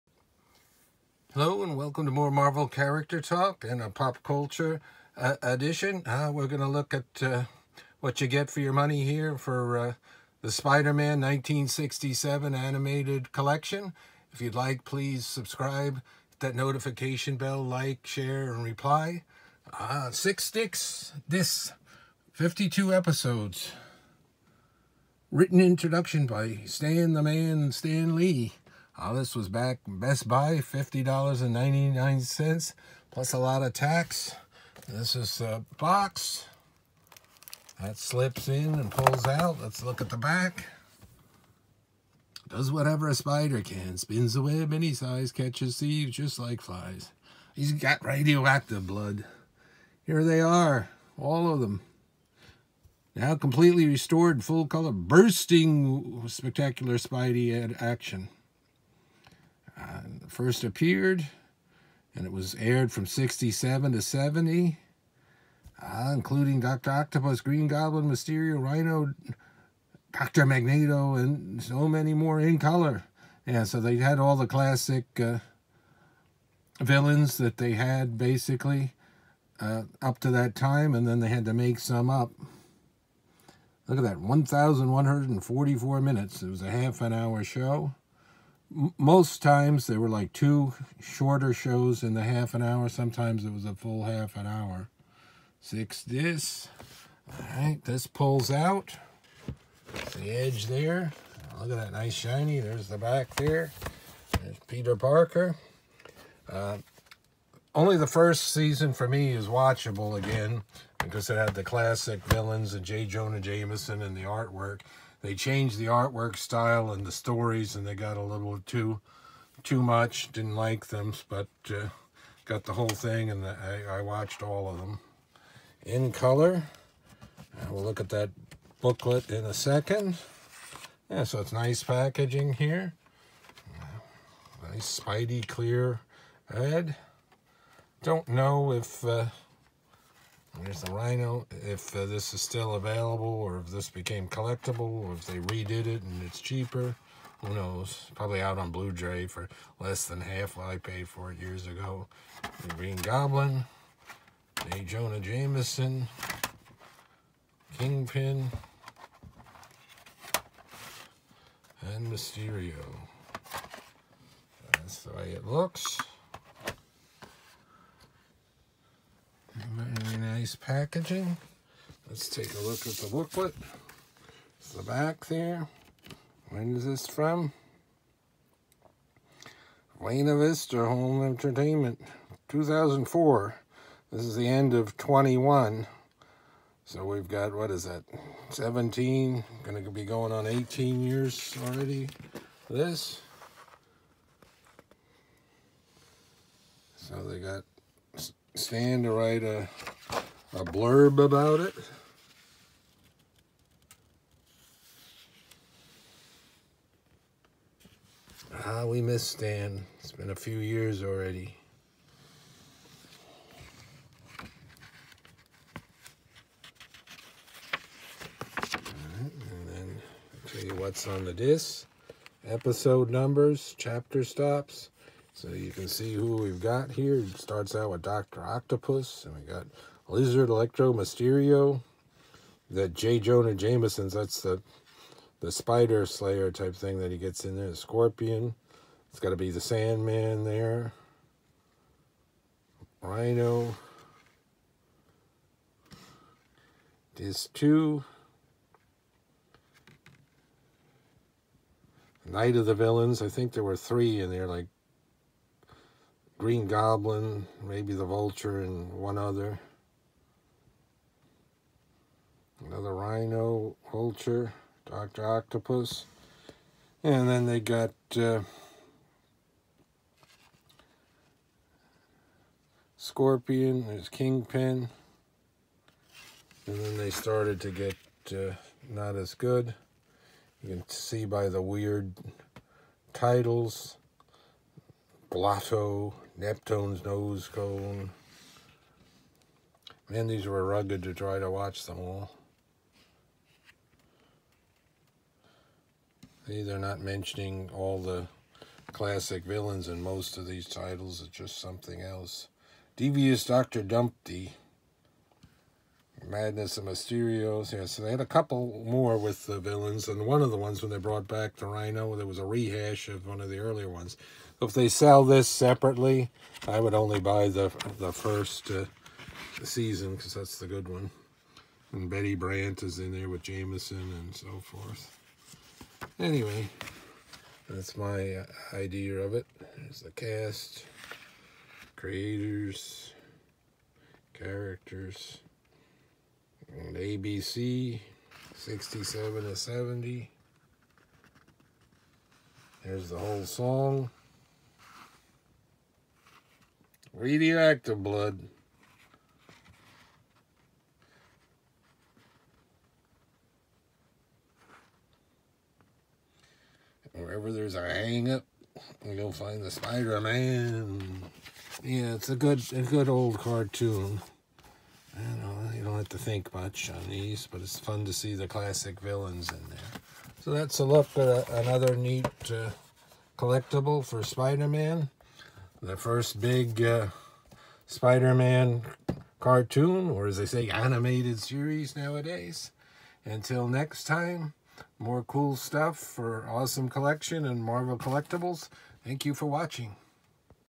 🕷 🕸 📺 Spider-Man The '67 Collection Cartoon DVD Set Marvel Comics 1967 Saturday Morning Cartoon 🕸🕷📺
📺🕸🕷 Spider-Man The '67 Collection Cartoon DVD Set Marvel Comics 1967 A look at the set 🕸 A great Saturday morning TV show that aired on ABC from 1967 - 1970 🕷📺 This DVD set came out in 2004. Amazing SpiderMan Cartoon Marvel MarvelCharacterTalk Part of the Awesome Collection of Marvel Comics and Collectibles. PopCulture Collectibles🗝📚🕷🕸🗝🕸📺🕷🕸📺🕸🕷🕸